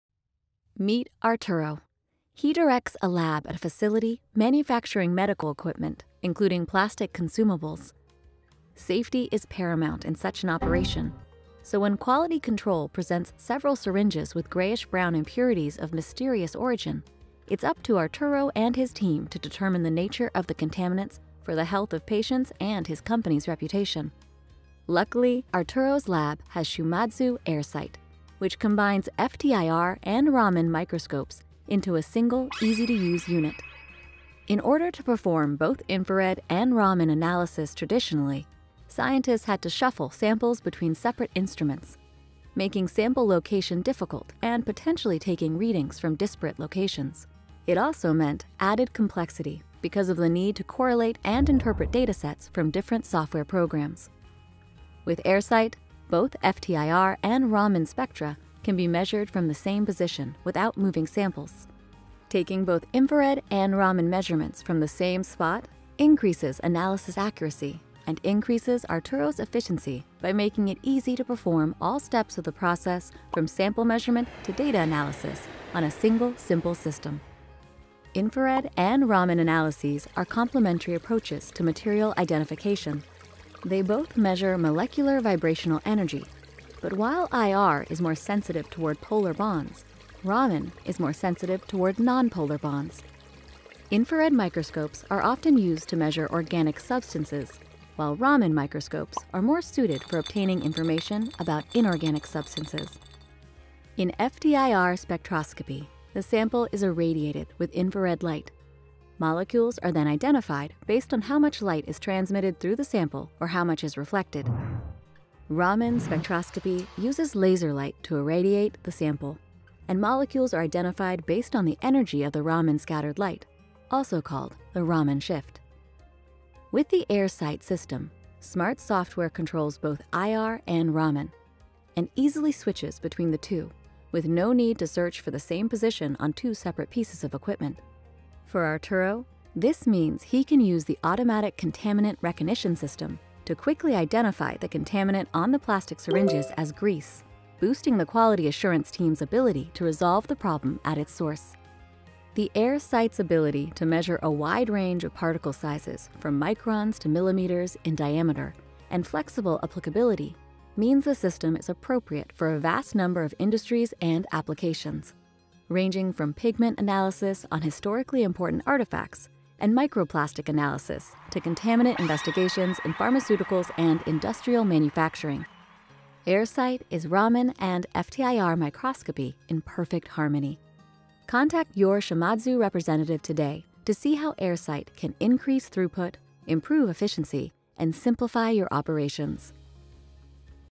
AIRsight Infrared/Raman Microscope
The AIRsight infrared Raman microscope improves the accuracy of qualitative analysis and operational efficiency by enabling both infrared and Raman measurements from the same position without moving the sample. This combined system saves valuable bench space while providing for easier operation with software that easily switches between infrared and Raman measurements via the same window.